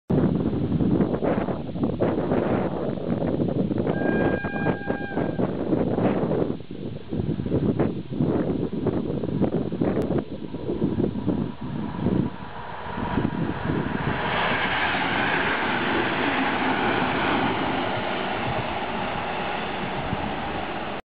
EN57 Regio jedzie z Kutna.(Rp1)
Przejazd na ul. Chełmońskiego.